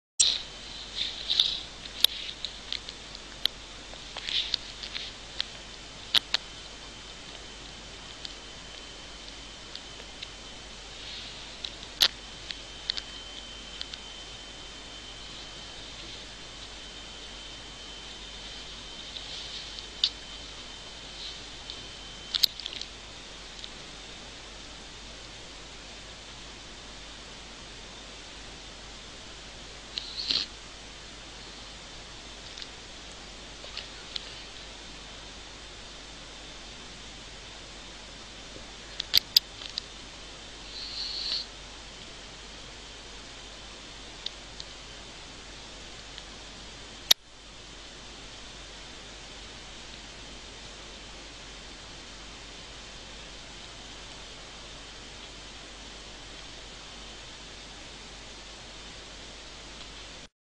cute cats
a cute mother cat and her kitten having a little nap. really cute